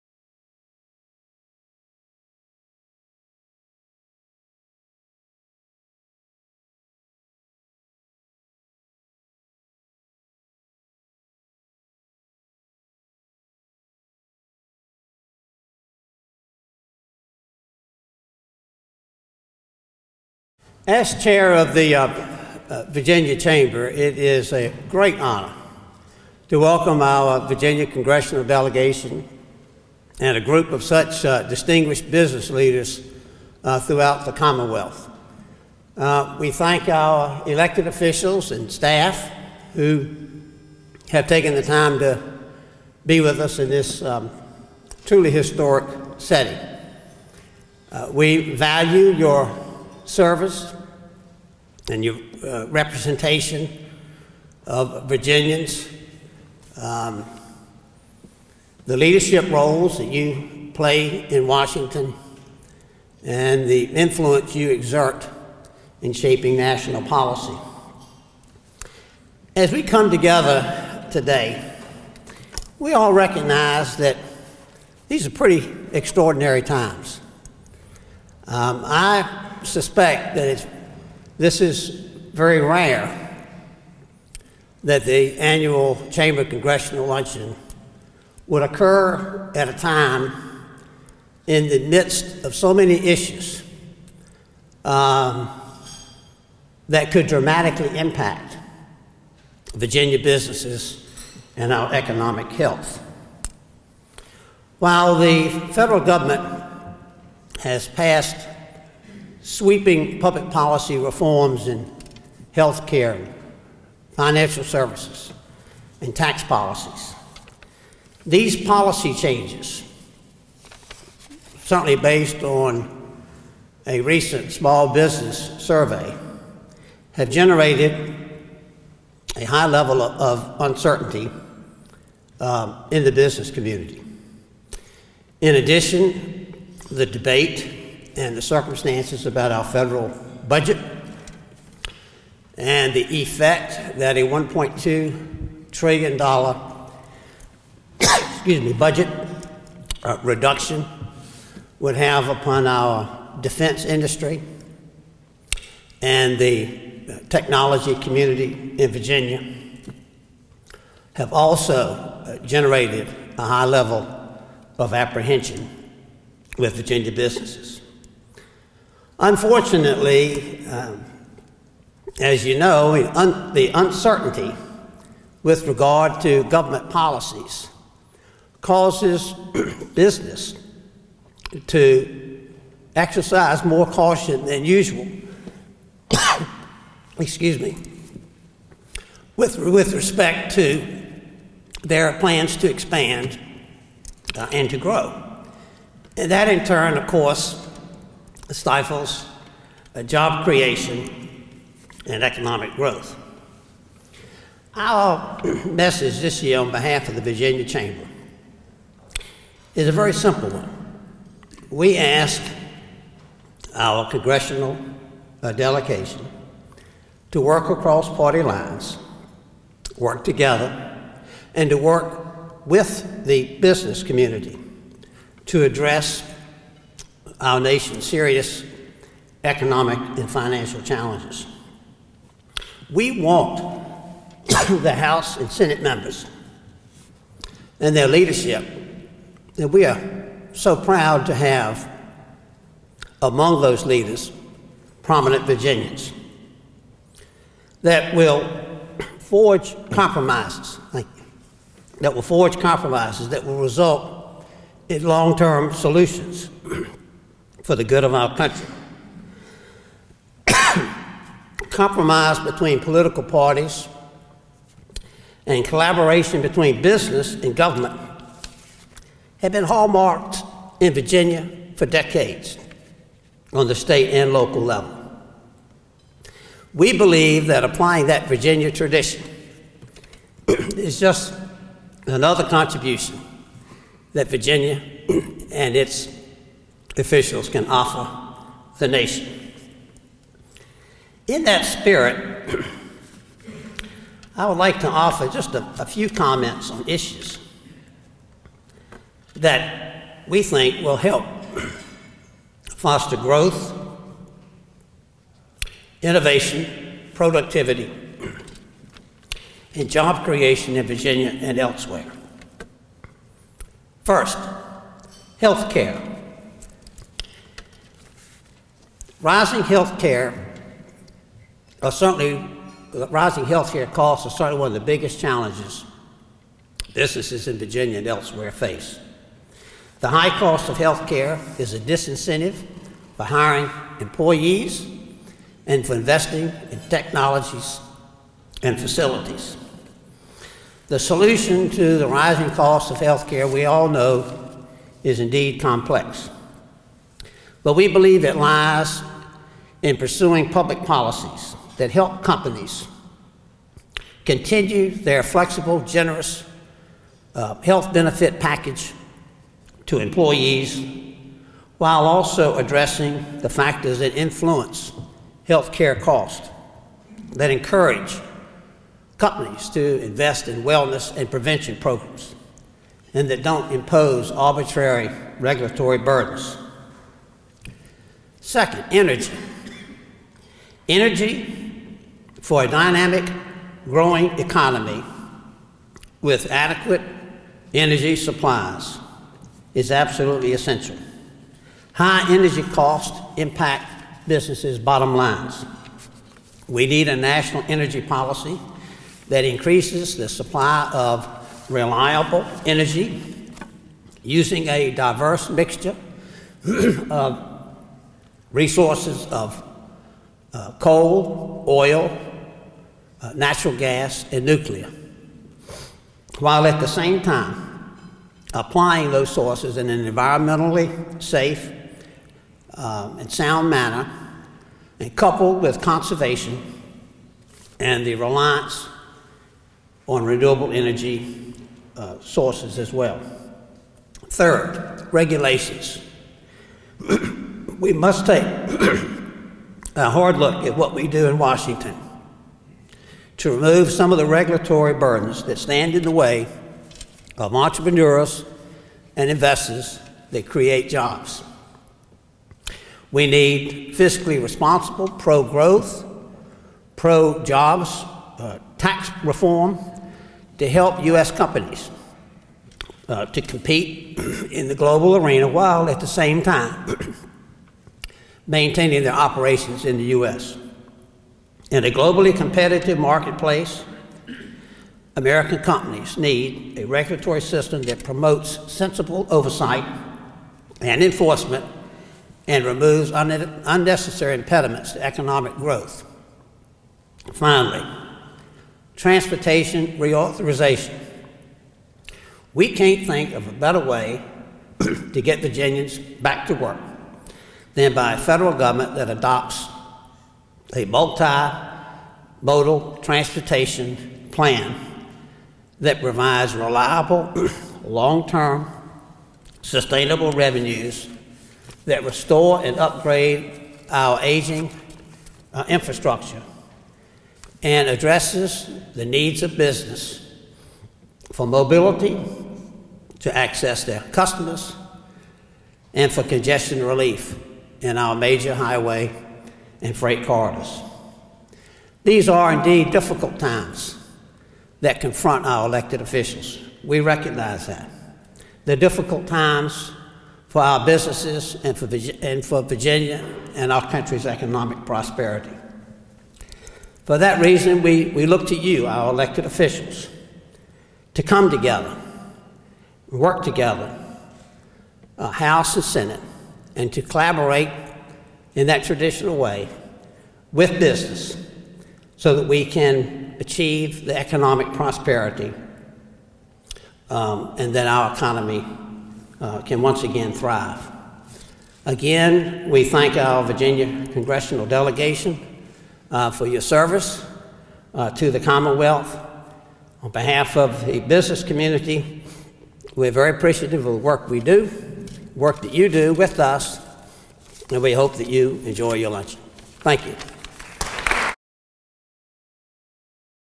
2011 Virginia Congressional Luncheon: Chairman Whitt Clement
Virginia Chamber of Commerce Chairman Whitt Clement's remarks at the 2011 Virginia Congressional Luncheon.